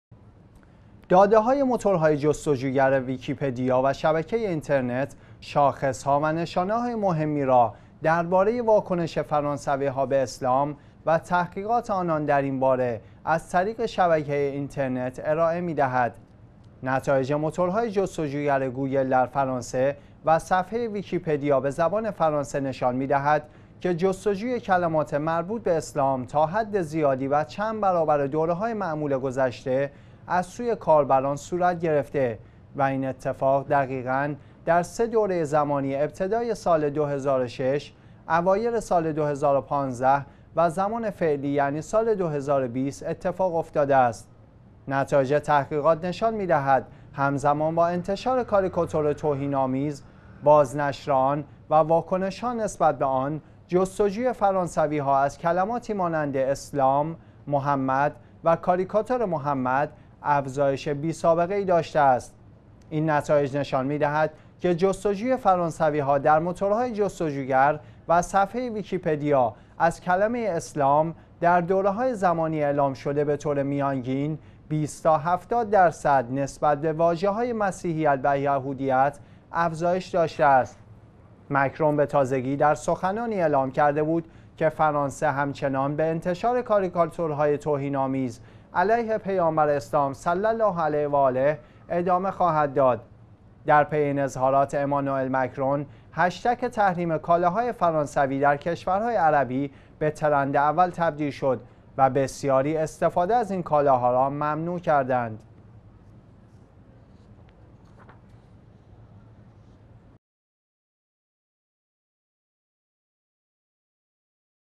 افزایش تحقیقات فرانسوی‌ها درباره دین اسلام
داده‌های موتور‌های جستجوگر، ویکی پدیا و شبکه اینترنت، شاخص‌ها و نشانه‌های مهمی را درباره واکنش فرانسوی‌ها به اسلام و تحقیقات آنان در این باره از طریق شبکه اینترنت ارائه می‌دهد.
نتایج موتور‌های جستجوگر گوگل در فرانسه و صفحه ویکی پدیا به زبان فرانسه نشان می‌دهد که جستجوی کلمات مربوط به اسلام تا حدزیادی و چند برابر دوره‌های معمول گذشته، از سوی کاربران صورت گرفته و این اتفاق دقیقا در سه دوره زمانی ابتدای سال ۲۰۰۶، اوایل سال ۲۰۱۵ و زمان فعلی یعنی سال ۲۰۲۰ اتفاق افتاده است.
نتایج تحقیقات نشان می‌دهد همزمان با انتشار کاریکاتور توهین آمیز، باز نشر آن و واکنش‌ها نسبت به آن، جستجوی فرانسوی‌ها از کلماتی مانند «اسلام»، «محمد» و «کاریکاتور محمد» افزایش بی سابقه‌ای داشته است.
این نتایج نشان می‌دهد که جستجوی فرانسوی‌ها در موتور‌های جستجو گر و صفحه ویکی پدیا از کلمه «اسلام» در دوره‌های زمانی اعلام شده به طور میانگین ۲۰ تا ۷۰ درصد نسبت به واژه‌های مسیحیت و یهودیت افزایش داشته است.
مکرون به تازگی در سخنانی اعلام کرده بود که فرانسه همچنان به انتشار کاریکاتور‌های توهین آمیز علیه پیامبر اسلام صلی الله علیه و آله ادامه خواهد داد.
در پی این اظهارات امانوئل مکرون، هشتگ «تحریم کالا‌های فرانسوی» در کشور‌های عربی به ترند اول تبدیل شد و بسیاری استفاده از این کالا‌ها را ممنوع کردند.